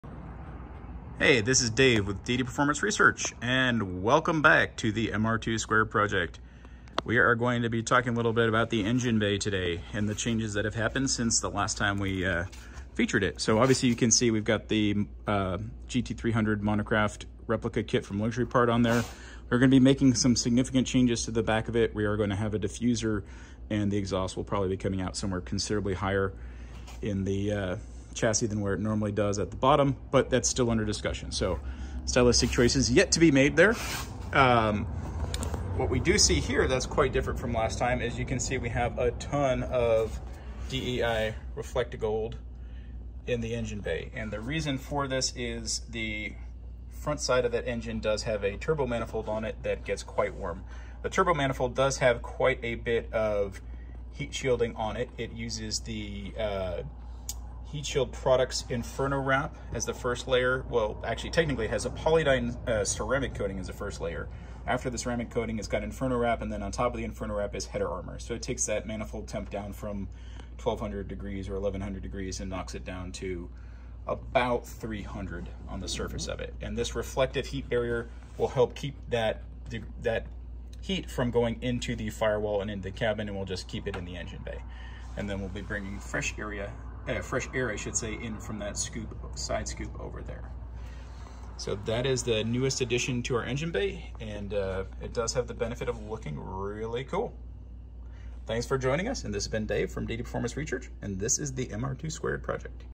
DDPR MR2 Squared Project 38
DDPR MR2 Squared Project 38 - This video covers the new DEI Gold heat management reflective coating in the engine bay